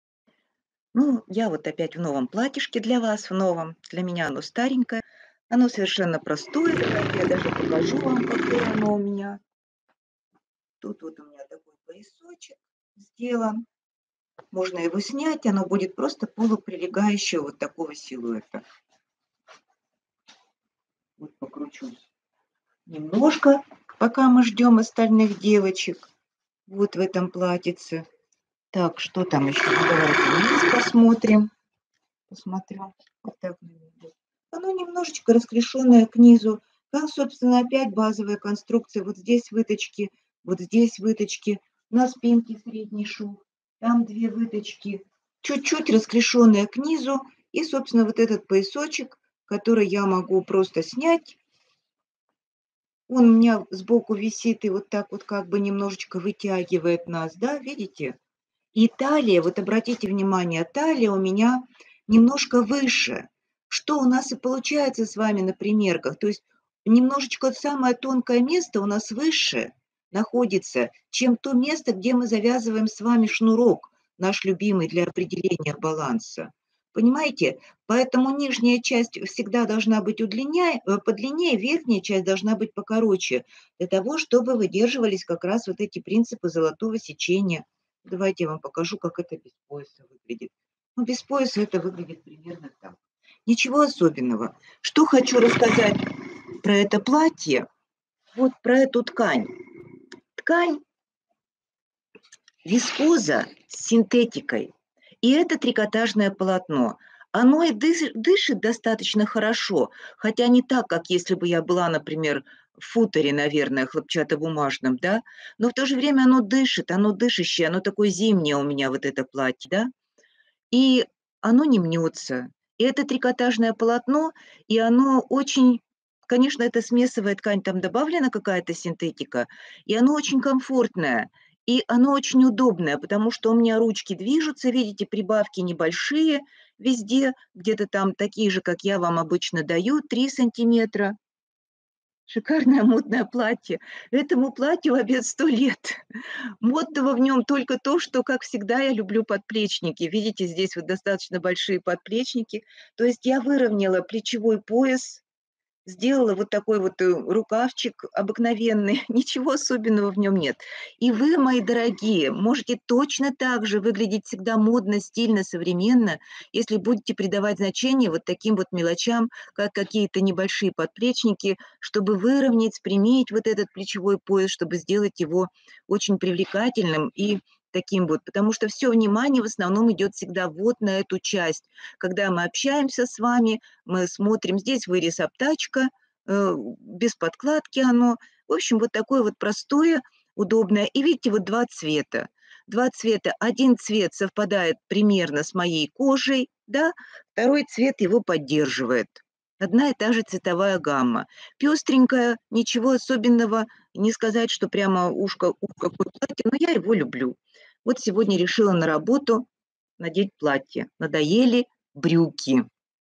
Ничего особенного - любимое платье
Давно заметила, что любимые платья, чаще всего простые по покрою. Это платье из вискозы с синтетикой. Не мнется - для меня это важно. Тело в нем дышит, хотя и не так, как в хлопковом. Мне в нем тепло, зимой тоже важно. Выполнено по обычной базовой выкройке. Немного расклешенное книзу. Удобный рукав, руки легко поднимаются.
Отрезная талия, длинный пояс свисает, зрительно вытягивая силует. Можно носить и без пояса.
Никокого сложного моделирования, подрезов здесь нет, т.к. ткань пестрая и это все просто бы потерялось, не было видно.
Плечевой пояс выровнен подплечниками. Общаясь с человеком, мы всегда смотрим на лицо. И в область зрения попадают горловина и плечевой пояс. Поэтому он так важен.
Вот так, ничего особенного, но платье любимое уже много лет!